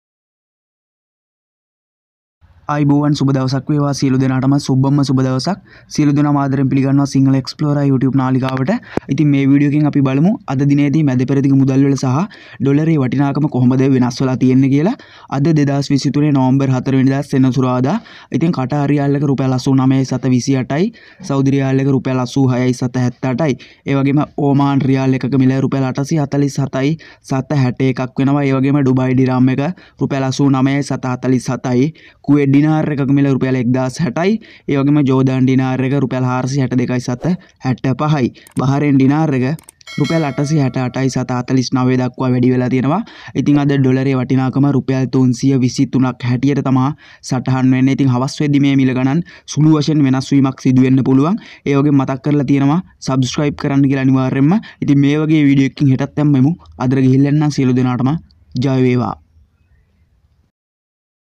අද ඩොලරය🇱🇰 උඩට⬆ exchange rates today | kuwait dinar |saudi riyal rate | dollar price in sri lanka
Sinhala explora💥🇱🇰

This video made for inform curreny Exchange rates today in sri lanka..

Including countries..

🟢 Kuwait dinar
🟢 Saudi riyal
🟢 Oman riyal
🟢 Jordan dinar
🟢 Qatar riyal
🟢 Bahrain dinar
🟢 Dubai dirham

Today dollar  323/=

Thanks ❤️